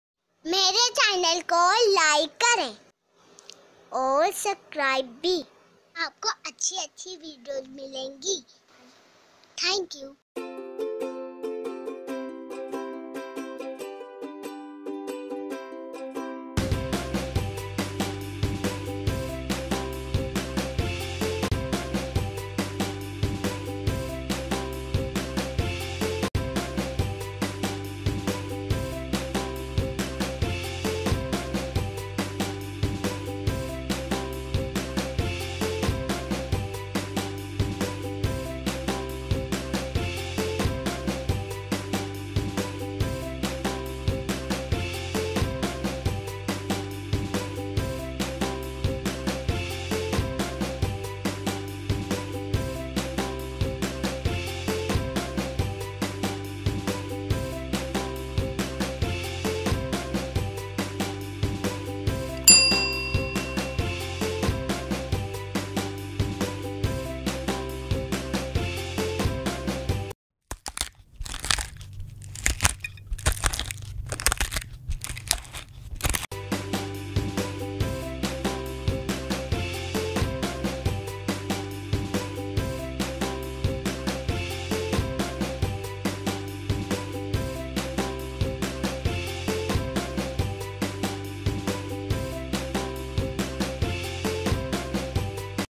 Dahi Bhala Masala Recipe | Homemade masala recipe | Kitchen with Maina
Today, I m sharing a  recipe of Dahi Bhalay Masala Powder at home.  There is  no good/healthy Food rather than homemade.Hope you would enjoy this recipe.